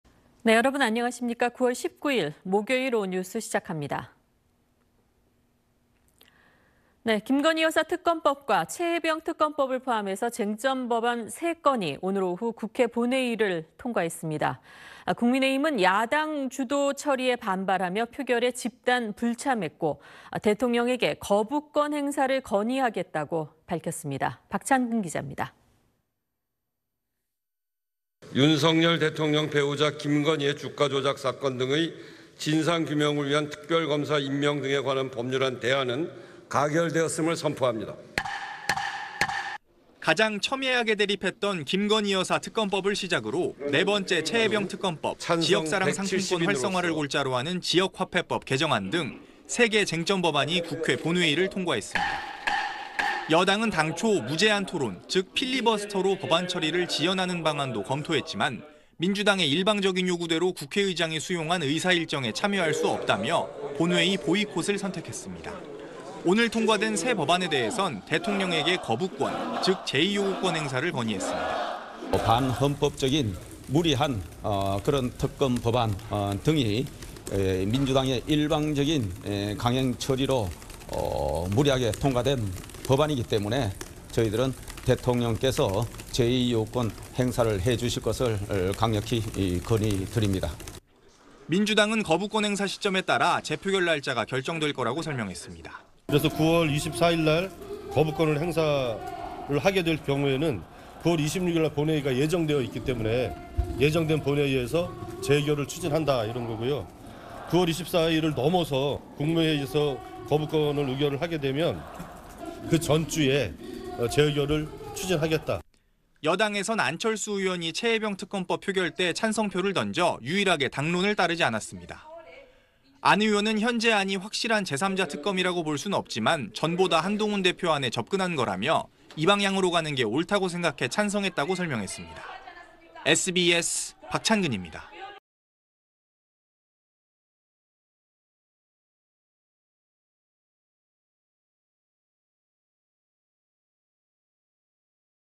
'김 여사·채 해병 특검법' 통과…여당 "거부권 건의" / SBS
〈앵커〉

김건희 여사 특검법과 채 해병 특검법을 포함해서 쟁점 법안 3건이 오늘(19일) 오후 국회 본회의를 통과했습니다. 국민의힘은 야당 주도 처리에 반발하며 표결에 집단 불참했고 대통령에게 거부권 행사를 건의하겠다고 밝혔습니다.

박찬근 기자입니다.

〈기자〉

[우원식/국회의장 : 윤석열 대통령 배우자 김건희의 주가조작 사건 등의 진상 규명을 위한 특별검사 임명 등에 관한 법률안 대안은 가결되었음을 선포합니다.]

가장 첨예하게 대립했던 김건희 여사 특검법을 시작으로 4번째 채 해병 특검법, 지역사랑 상품권 활성화를 골자로 하는 지역화폐법 개정안 등 3개 쟁점 법안이 국회 본회의를 통과했습니다.

여당은 당초 무제한토론, 즉 필리버스터로 법안 처리를 지연하는 방안도 검토했지만, 민주당의 일방적인 요구대로 국회의장이 수용한 의사일정에 참여할 수 없다며 본회의 보이콧을 선택했습니다.

오늘 통과된 세 법안에 대해선 대통령에게 거부권, 즉 재의요구권 행사를 건의했습니다.

[추경호/국민의힘 원내대표 : 반헌법적인, 무리한 그런 특검법안 등이 민주당의 일방적인 강행 처리로 무리하게 통과된 법안이기 때문에 저희들은 대통령께서 재의요구권 행사를 해주실 것을 강력히 건의드립니다.]

민주당은 거부권 행사 시점에 따라 재표결 날짜가 결정될 거라고 설명했습니다.

[윤종군/민주당 원내대변인 : 9월 24일 거부권을 행사를 하게 될 경우에는 9월 26일 본회의 예정돼 있기 때문에 예정된 본회의에서 재의결을 추진한다 이런 거고요. 9월 24일 넘어서 국무회의에서 거부권을 의결을 하게 되면 그 전주에 재의결을 추진하겠다.]

여당에서는 안철수 의원이 채 해병 특검법 표결 때 찬성표를 던져 유일하게 당론을 따르지 않았습니다.

안 의원은 "현재 안이 확실한 제3자특검이라고 볼 순 없지만 전보다 한동훈 대표안에 접근한 것"이라며 "이 방향으로 가는 게 옳다고 생각해 찬성했다"고 설명했습니다.

(영상취재 : 박현철, 영상편집 : 남 일)